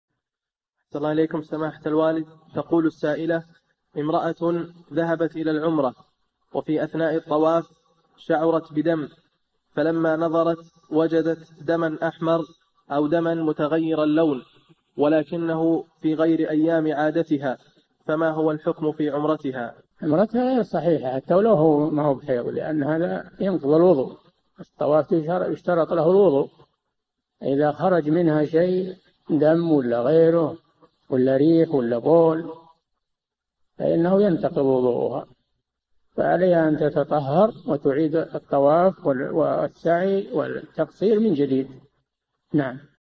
[2442] امرأة خرج منها دم أثناء الطواف فما حكم عمرتها - الشيخ صالح الفوزان
"[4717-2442] أَحْسَنَ اَللَّهُ إِلَيْكُمْ سَمَاحَةَ الْوَالدِ، تقول السائلة: امرأة ذهبت إلى العمرة وفي أثناء الطواف شعرت بدم فلما نظرت وجدت دما أحمر أو دما متغير اللون، ولكنه في غير أيام عادتها، فما هو الحكم في عمرتها؟
الجَوَابُ: عمرتها غير صحيحة حتى ولو ما هو بحيض؛ لأن هذا ينقض الوضوء، الطواف يشترط له الوضوء، فإذا خرج منها شيء دم أو غيره أو ريح أو بول، فإنه ينتقض وضوؤها، فعليها أن تتطهر وتعيد الطواف والسعي والتقصير من جديد.

للاشتراك في قناة الشيخ صالح الفوزان

للاستفسار والإقتراحات والدعم
رئيس الفريق
لمشروع كبار العلماء